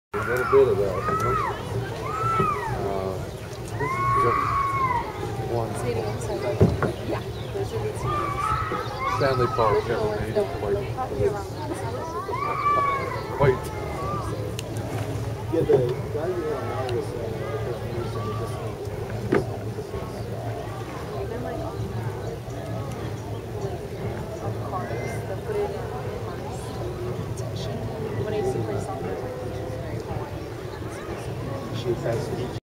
Hawaii Vacation 2011, Canoe Ride Polynesian Culture Center
A canoe ride just before the pageant.